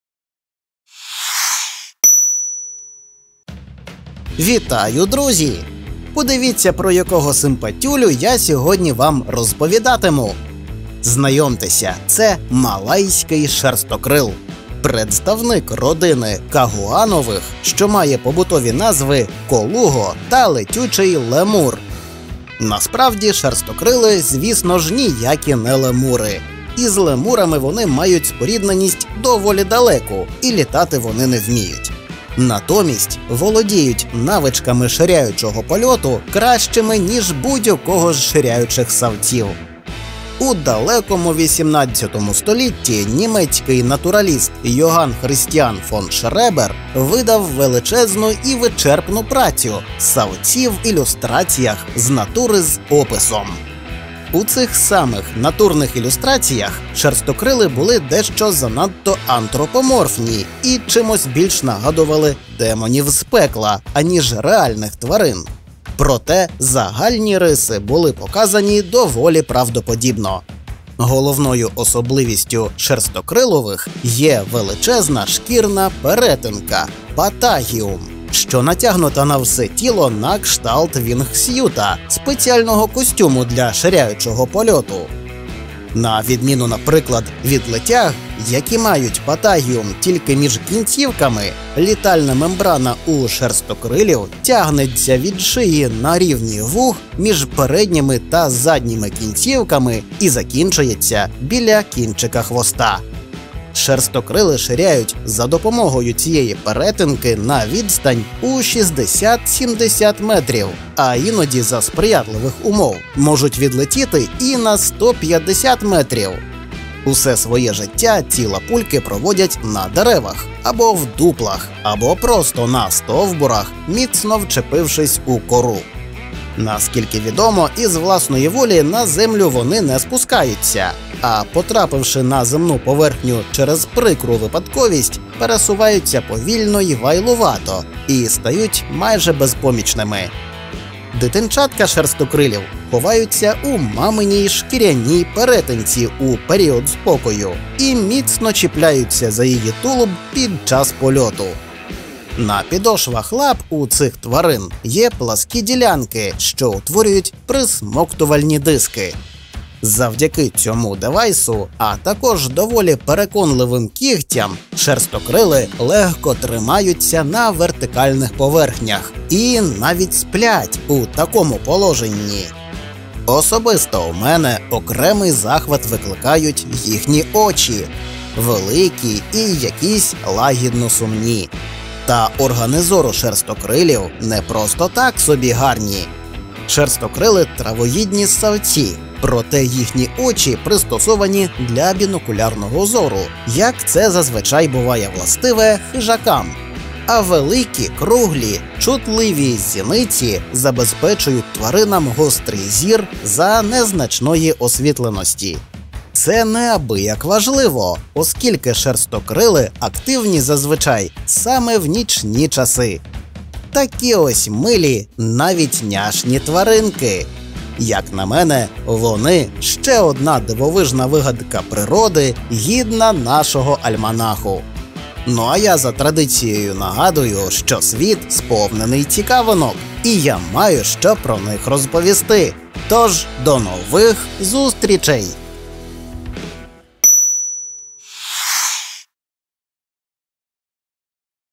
Дива природи. Явища навколо нас. Шерстокрили
Сьогодні Вчитель розповість про дивовижних ширяючих ссавців — шерстокрилів. Подробиці у відео!

Під час створення фільму використані такі матеріали:
Image by:
Goldfuss, Georg August; Schreber, Johann Christian Daniel; Wagner, Andreas Johann Public Domain,

As well as photos and videos according to Premium license (Unlimited use without attribution) Freepik.com